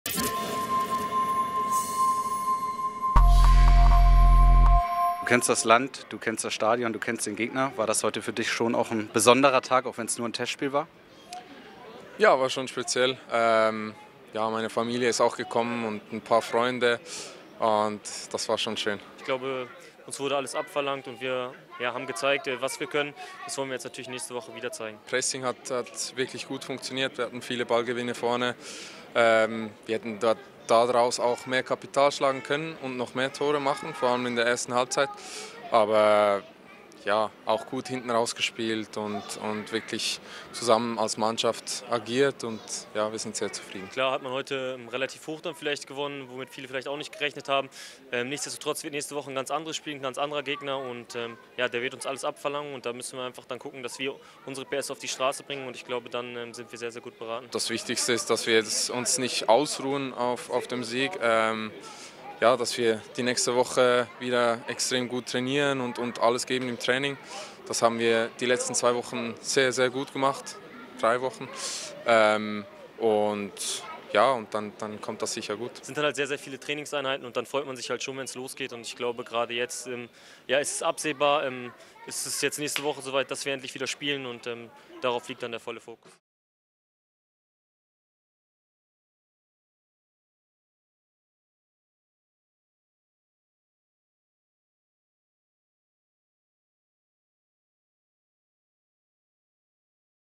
DIE INTERVIEWS NACH DEM TESTSPIEL I FC Basel vs. HSV
Nach dem 5:1-Testspielerfolg gegen den FC Basel standen Miro Muheim und Mo Heyer bei uns am Mikrofon und bilanzierten die Partie gegen den Schweizer Vize-Meister.